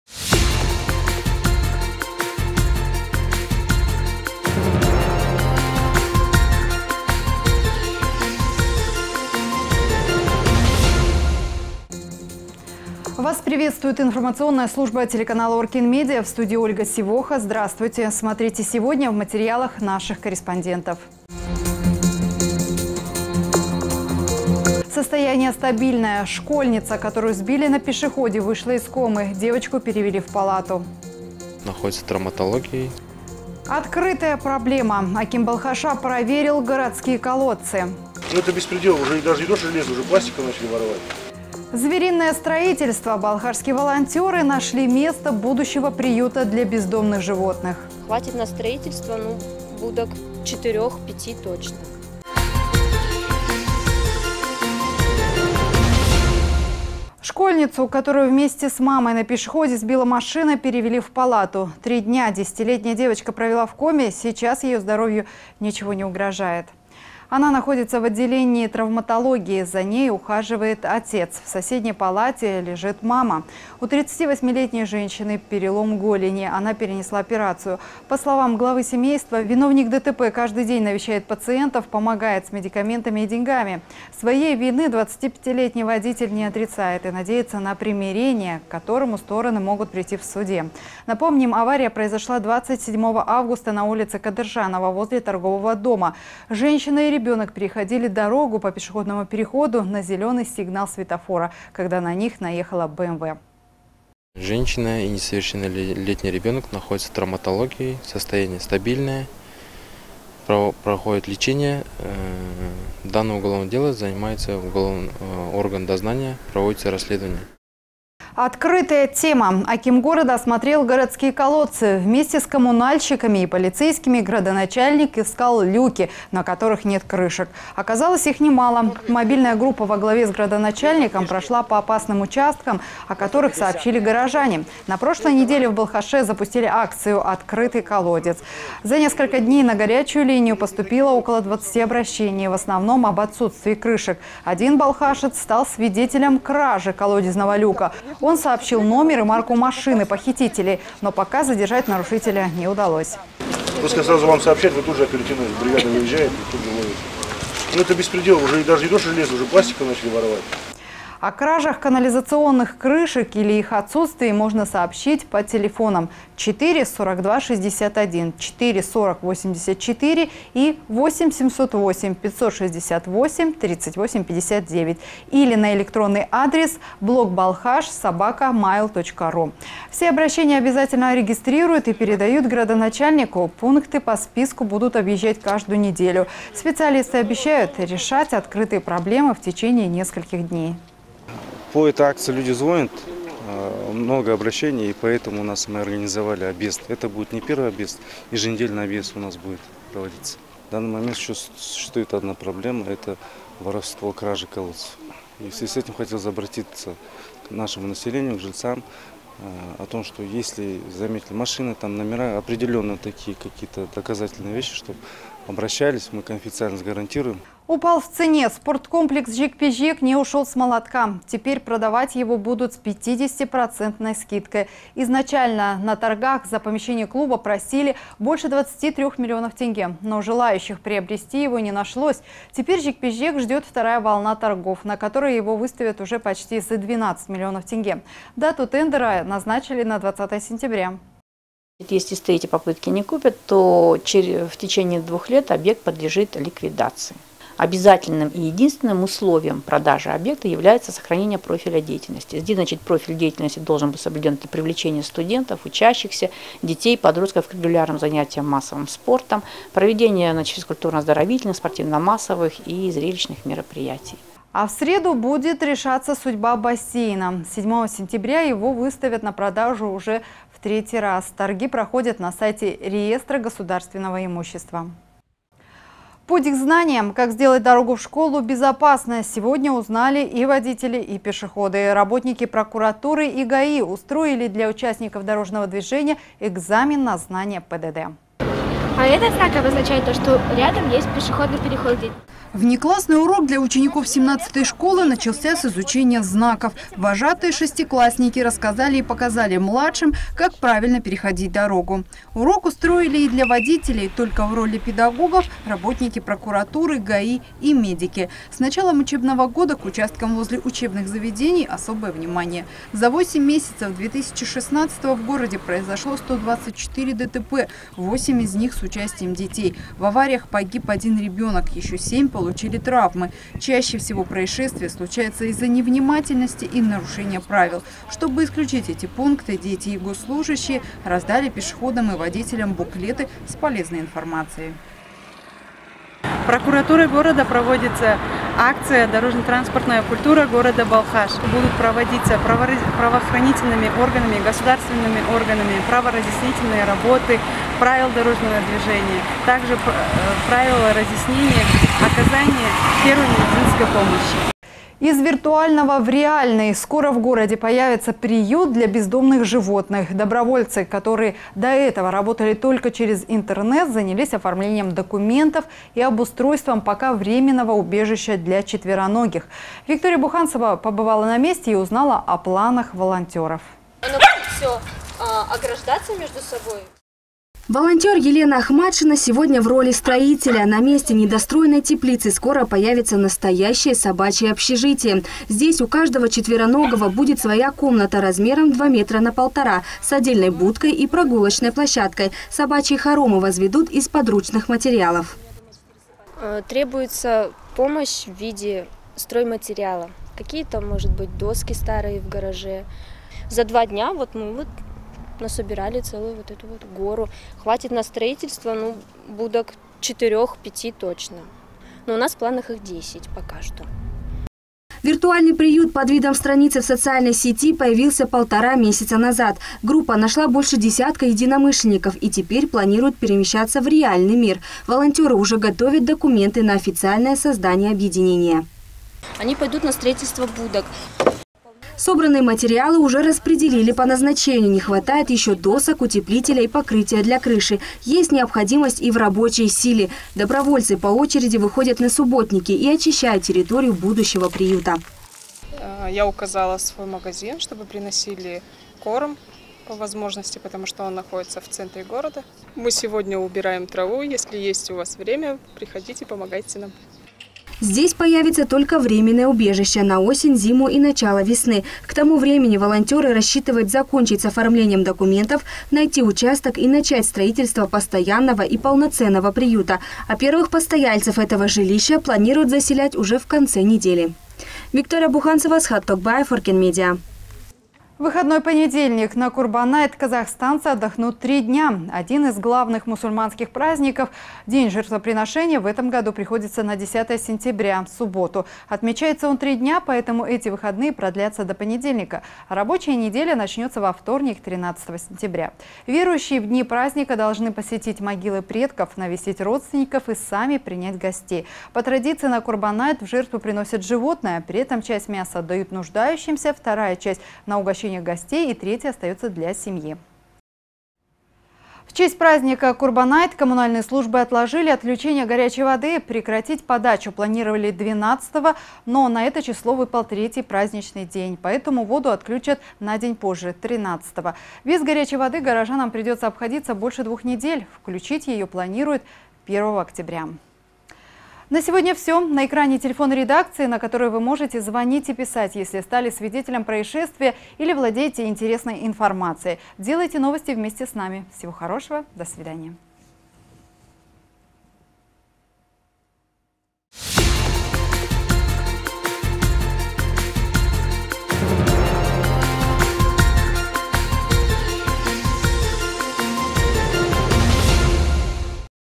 Новости Балхаша 05.09.2016
Открытая проблема! Аким Балхаша  проверил городские колодцы
Балхашские волонтеры нашли место будущего приюта для бездомных животных